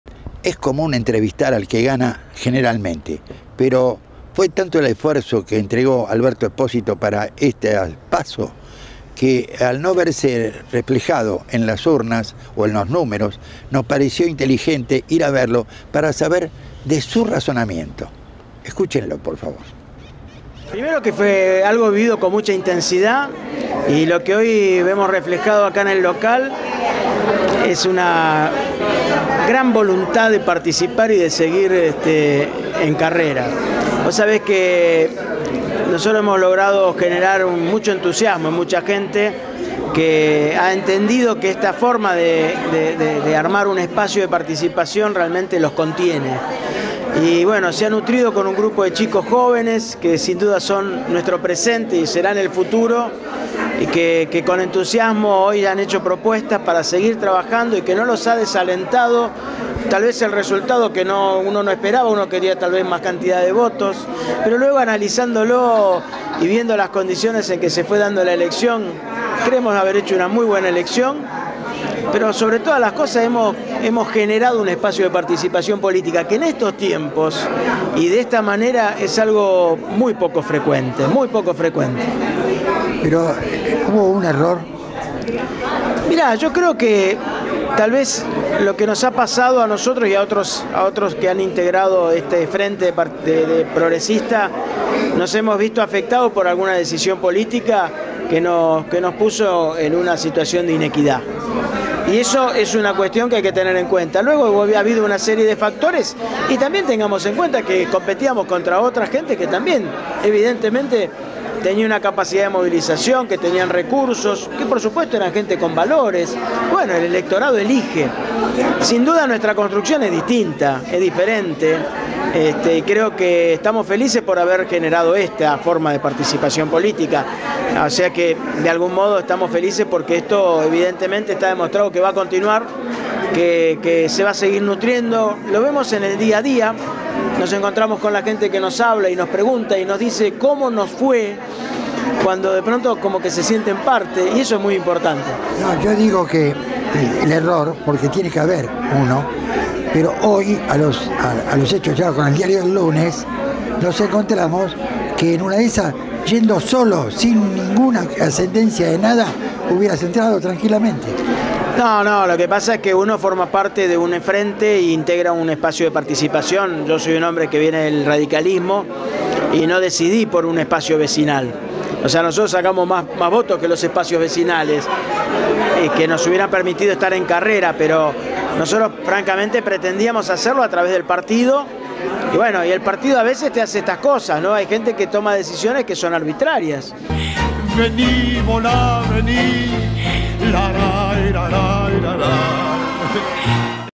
Florida: Alberto Exposito y su balance post elecciones
Pasado las elecciones de las PASO Alberto Exposito hace un balance de las elecciones de las PASO en el Partido de Vicente López